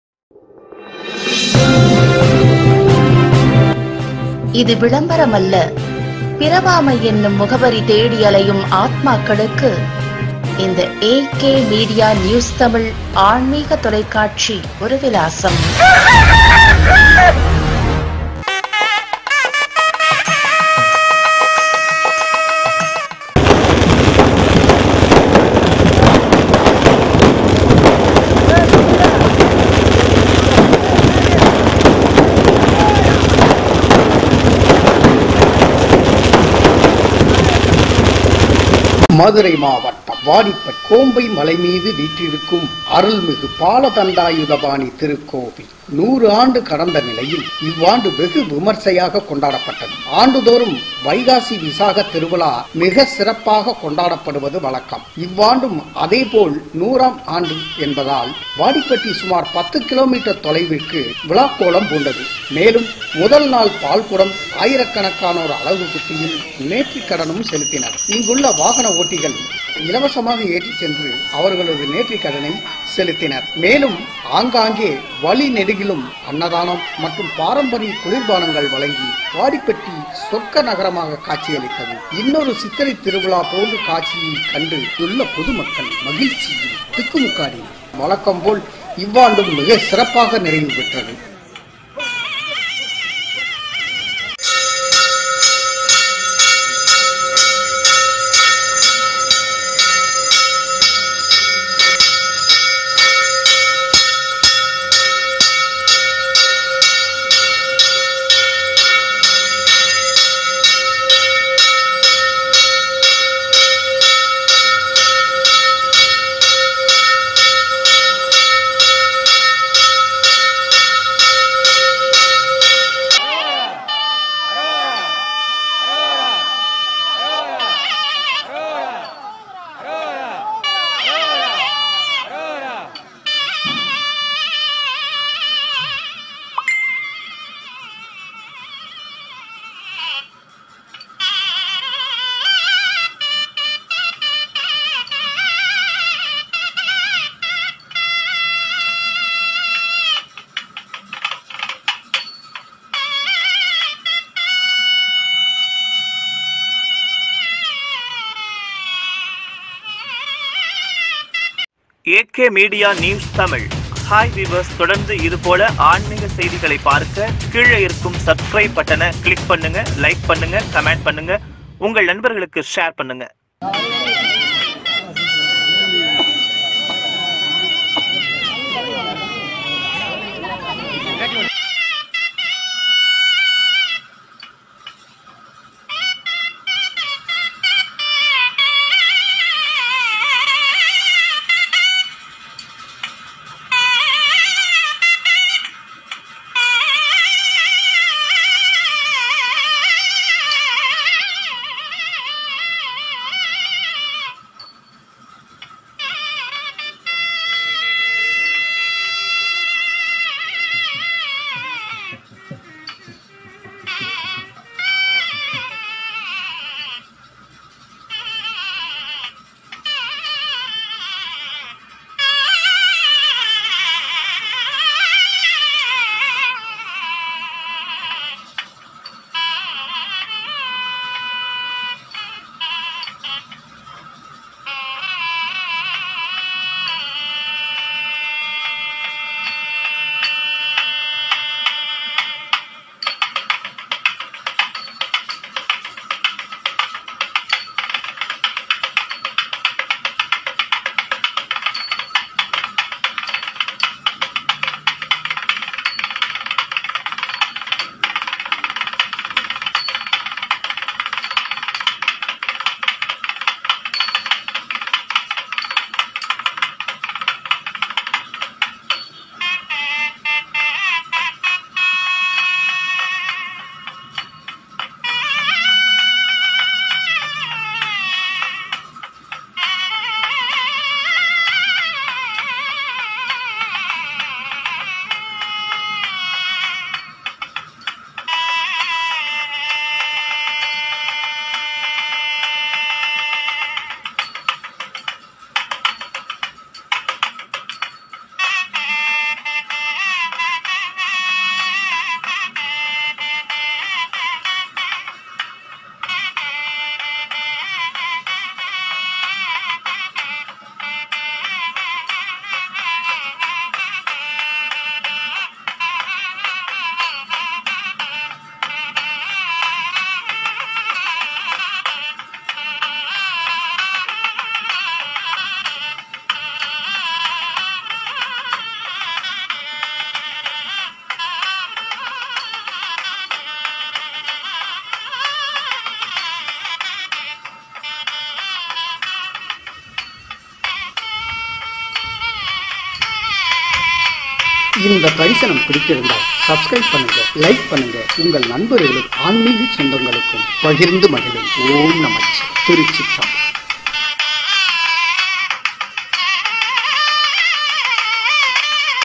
வாடிப்பட்டி வைகாசி பெருந்திருவிழா மகிழ்சியில் திக்குமுக்காடிய பொதுமக்கள் இன்னொருசித்திரை திருவிழாவா?
வாடிப்பட்டி வைகாசி பெருந்திருவிழா சுமார் 10 கிலோமீட்டர் தொலைவிற்கு விழாக்கோலம் மகிழ்சியில் திக்குமுக்காடிய பொதுமக்கள் இன்னொரு சித்திரை திருவிழாவா?